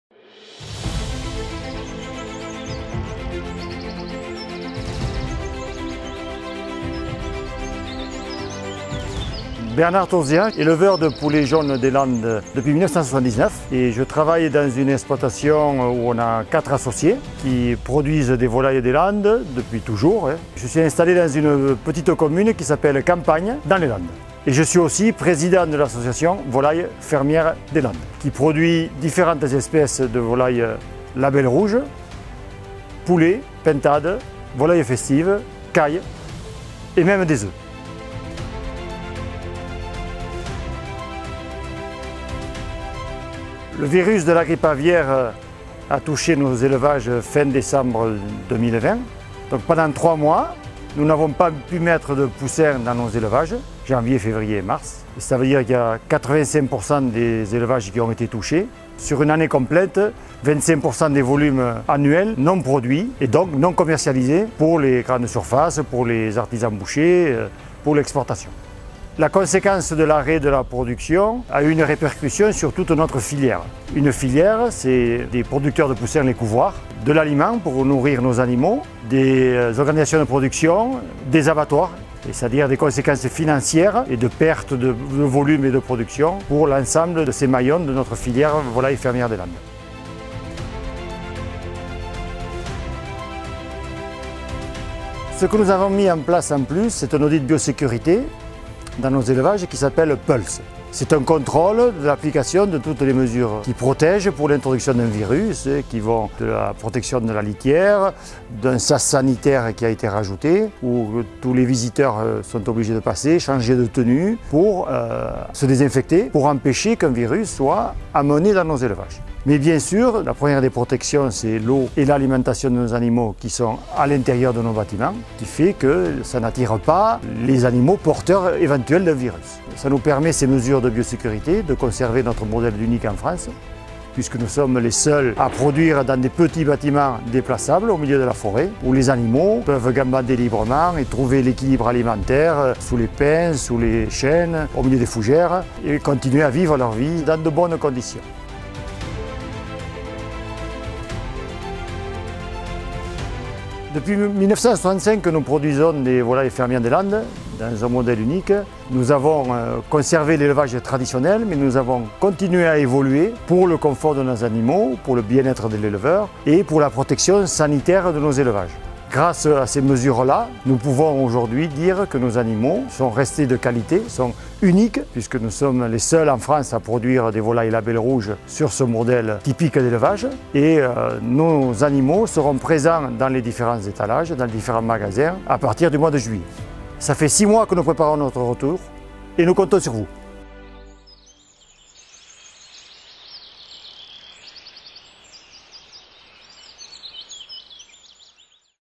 Bernard TAUZIA - Président de l'Association des Volailles Fermières des Landes et éleveur landais
Grâce à la bio-sécurité, les Volailles Fermières des Landes préservent leur mode d'élevage unique en France : en liberté, et avec de petits bâtiments mobiles dans le forêt des Landes. Poulets, pintades, volailles festives, cailles, les Volailles Fermières des Landes Label Rouge signent leur grand retour dans tous les rayons des commerces de France et à l'export. Rencontre avec Bernard Tauzia, Président de l'Association des Volailles Fermières des Landes.